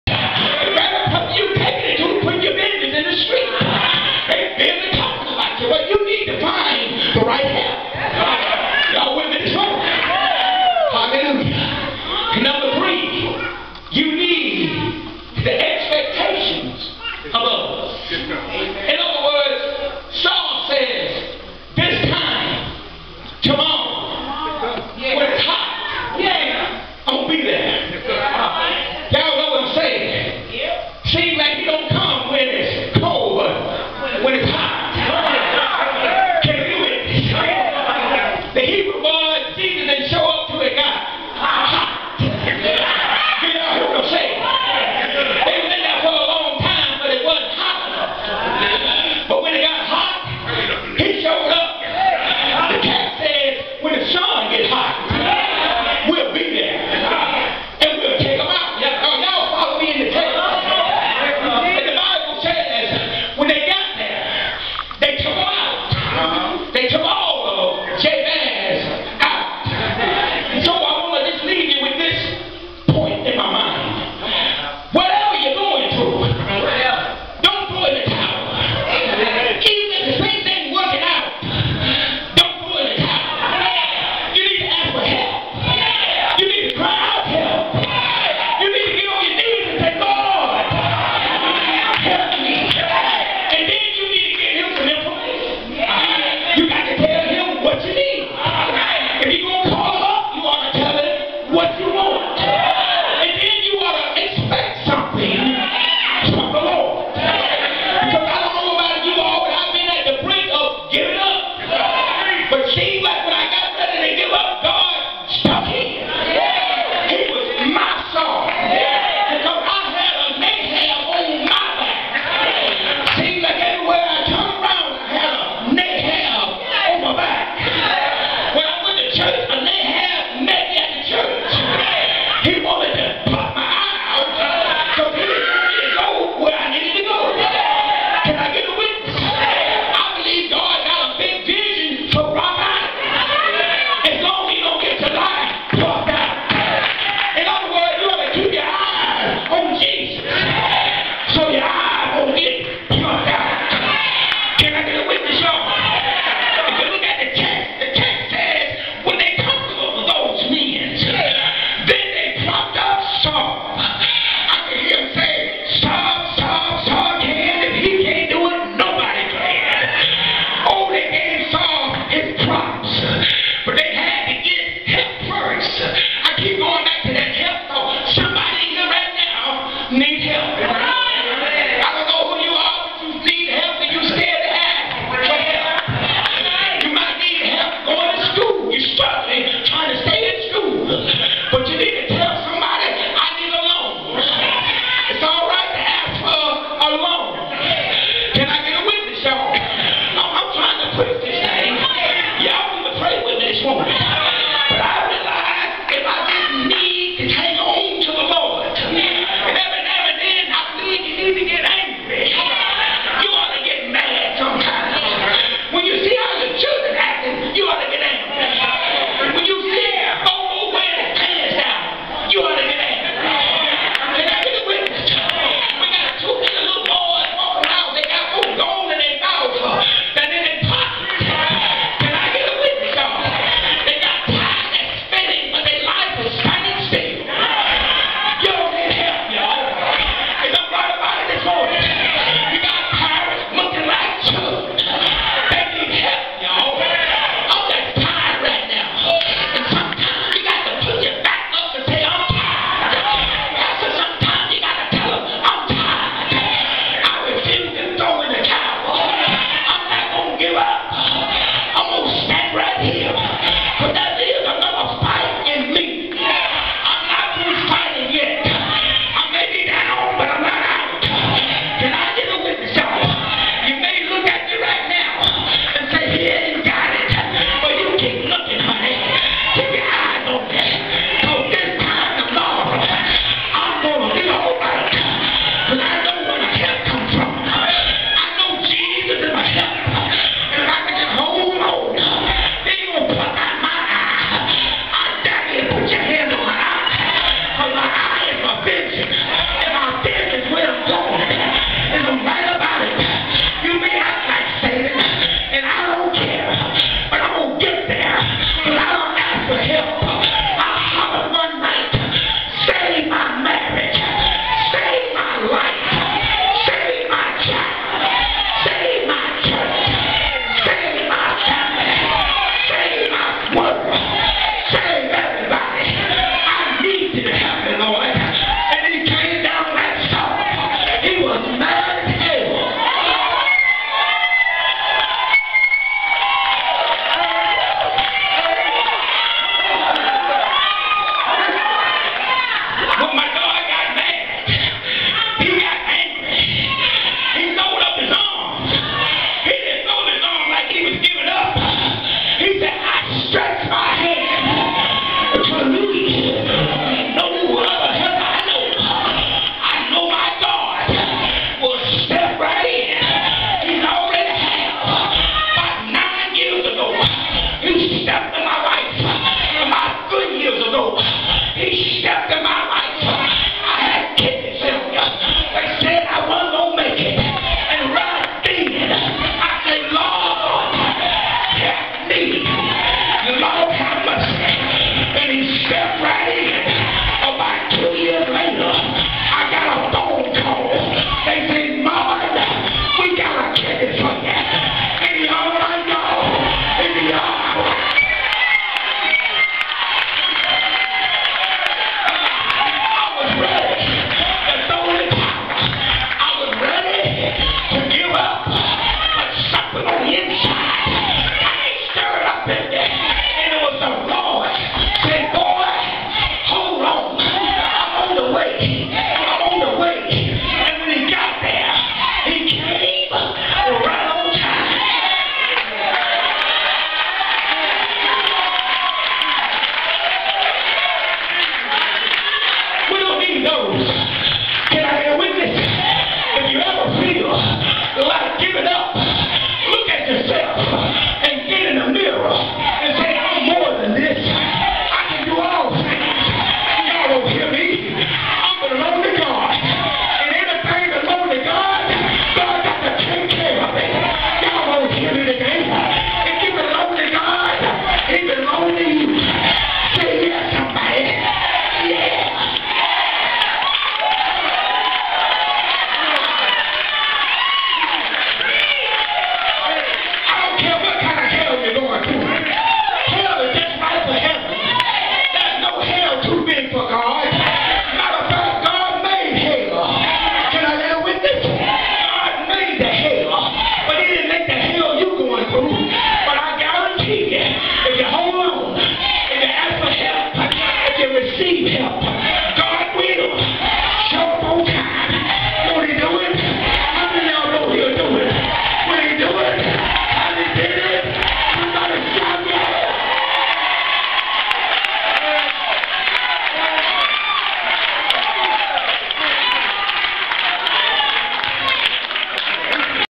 102812b-3
Rock Island Baptist Church in Chattanooga, TN celebrates the 12th Pastoral Anniversary of Pastor Ronald B. Cook, Sr. and First Lady Cook, Sr. on Sunday, October 28, 2012. Guests for the afternoon service was Pastor and First Lady Waymon Martin and Center Hill Baptist Church from Atlanta, GA. Part 3 of 4.